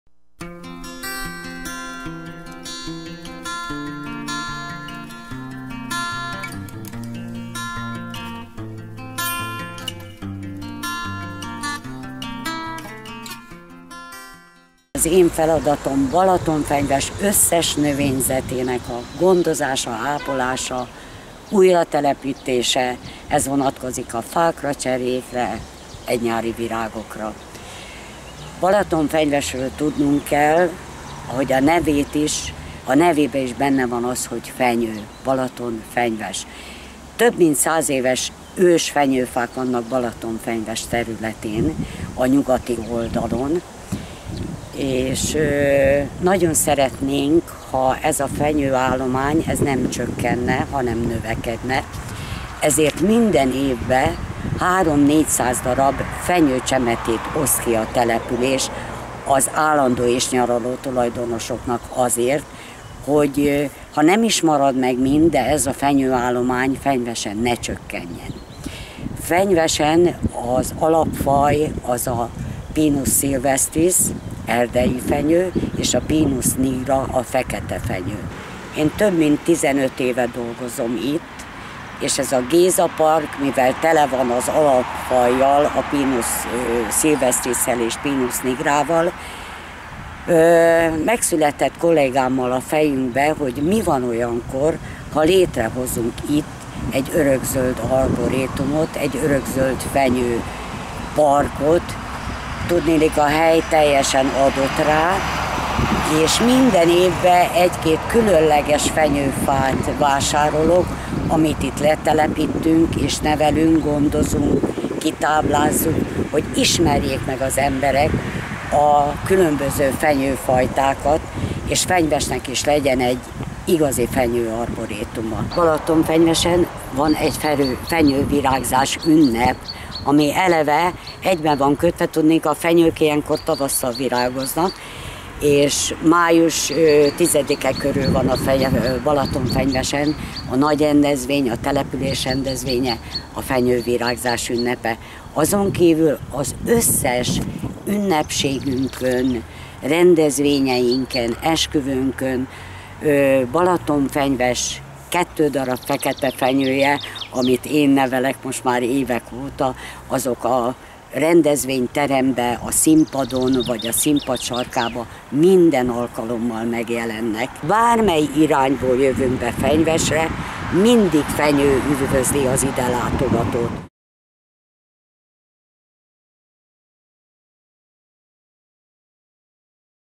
YouTube code: NAT_01
Title: HA FENYO, AKKOR BALATONFENYVES! (IF YOU'RE PINE TREE, THEN BALATONFENYVES!)
Contest category: 3- NATURAL ENVIRONMENT
Country: Hungary
Author: Dákai Sára, Rimmel Fruzsina
School: Balatonfenyvesi Fekete István Általános Iskola